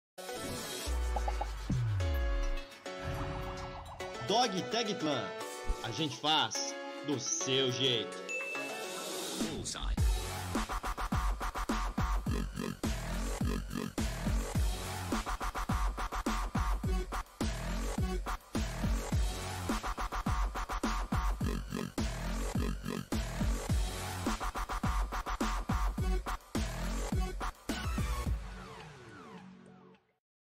DOG TAG PLACA PLAQUETA MENSAGEM PAI PARA FILHO
DESCRIÇÃO DO PRODUTO:

A Dog Tag de pais para filhos, é o modelo clássico de DogTag, sendo o famoso "raíz", com chapa de aço gravada com o nome de seu filho ou filha. O diferencial, é que esta dog tag será algo que você pode presentear, e seu filho ou filha jamais irá esquecer, pois estará sempre no pescoço com ela!

O produto, possui um conjunto de corrente de bolinha de 62cm de comprimento e mais um de 5cm com fecho do tipo canoa (trava de junção / terminal da corrente) que acompanha a dog tag.

COMO PERSONALIZAR?

Nós da DogTagClan, recomendamos a personalização da seguinte forma:

1- PERSONALIZAÇÃO COM NOME:

Você pode personalizar sua Dog Tag com o PRIMEIRO NOME da pessoa que será presenteada.
2- PERSONALIZAÇÃO ESCOLHENDO A CORRENTE:

Você também é livre para escolher o tipo de corrente que deseja, para acompanhar sua dog tag, sendo eles:
Aço Inox  - NÃO ENFERRUJA.
Dourada.
Black Piano.
Latão.
Comum.
3- PERSONALIZAÇÃO ESCOLHENDO CAIXA PARA PRESENTE:

Caso prefira complementar sua personalização com uma caixa para dar de presente ou para você mesmo, a DogTagClan disponibiliza algumas opções para que você possa escolher para seu produto no momento da compra. Caso não queira, basta não selecionar nenhuma das opções disponíveis.
7- POSSO PERSONALIZAR GRAVANDO FRENTE E VERSO?

Neste modelo, é gravado apenas na frente da Dog Tag, não sendo possível a gravação na parte de trás da mesma, deixando a parte traseira da tag em aço. Porém, é possível realizar a gravação na frente e no verso, através do modelo DOUBLE de Dog Tag!